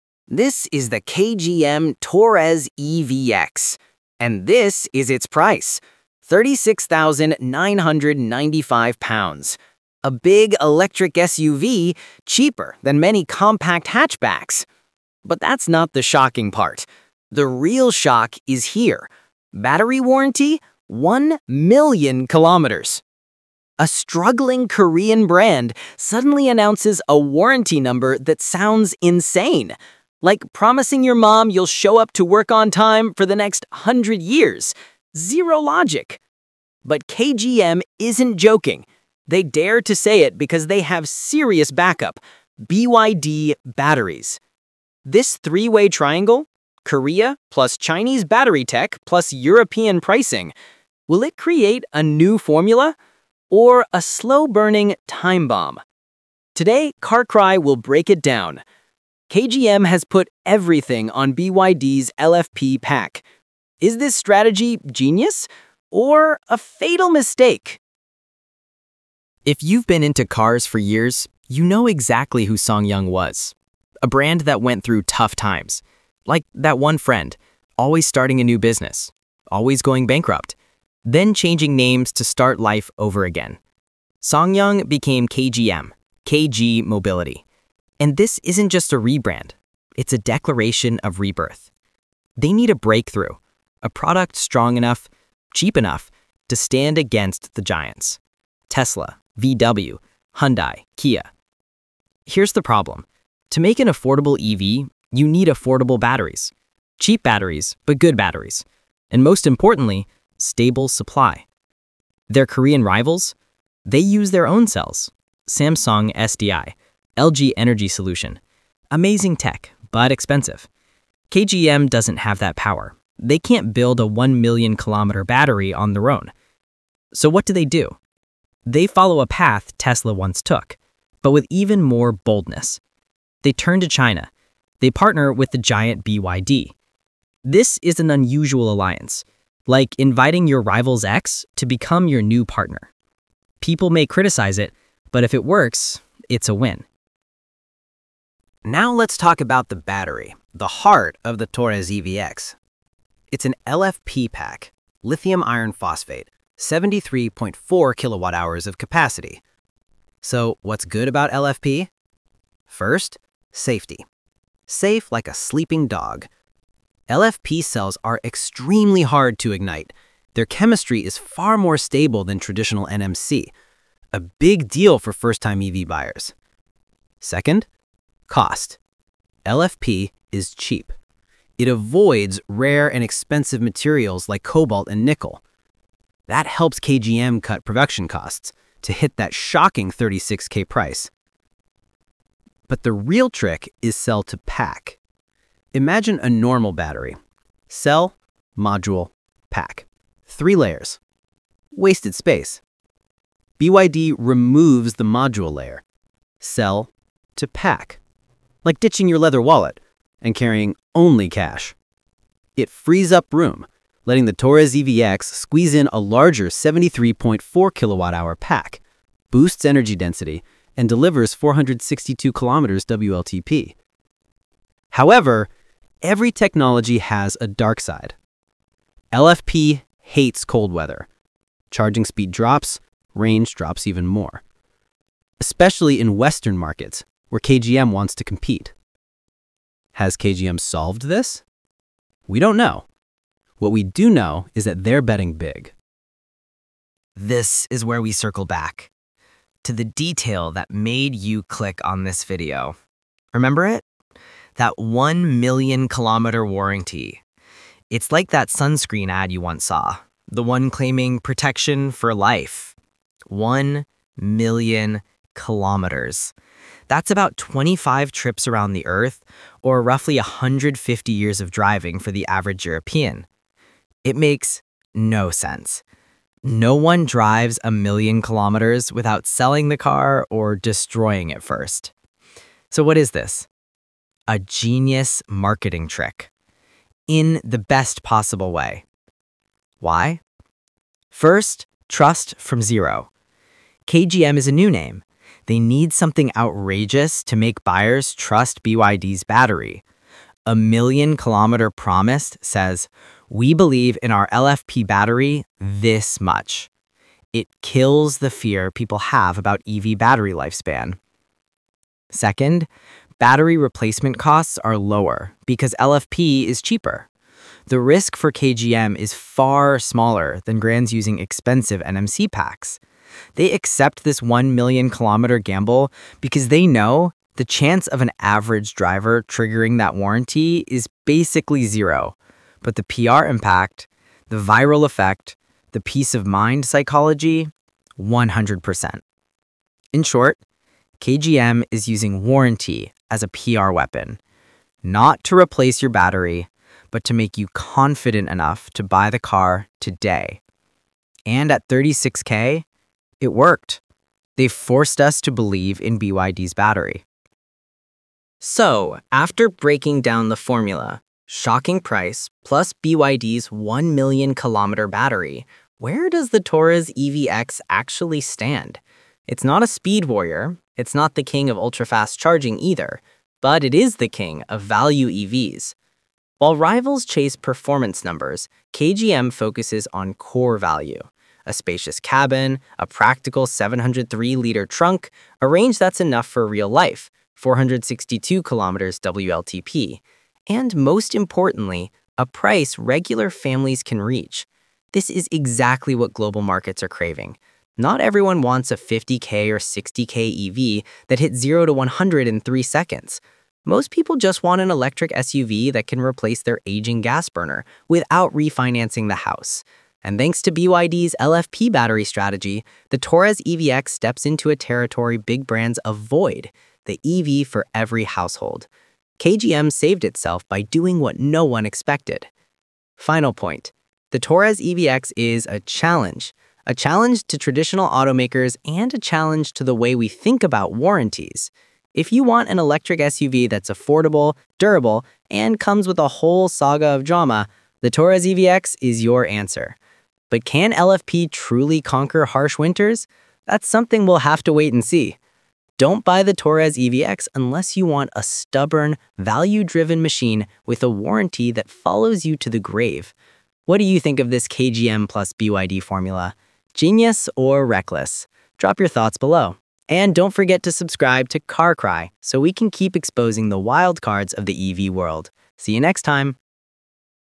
KGM Torres EVX: £36,995… and a 1,000,000-KM BYD Battery Warranty?! Genius or Marketing Madness?
The KGM Torres EVX just dropped into the UK market with a price that undercuts the Tesla Model Y — and a BYD LFP battery backed by an unbelievable 1,000,000-km warranty. Is this the bold formula that revives KGM (formerly SsangYong), or is it a high-stakes marketing gamble?

In this video, Carcry breaks down how the BYD 73.4 kWh LFP Cell-to-Pack battery works, why it’s safer and cheaper, and how it helps the Torres EVX hit such an aggressive price point. We also analyse whether a million-kilometre warranty is realistic in real-world European climates, and how the Torres EVX stacks up against the Model Y in value, durability, and long-term ownership.

If you’re curious whether KGM’s comeback story is real — or just loud marketing — this is the deep dive you don’t want to miss.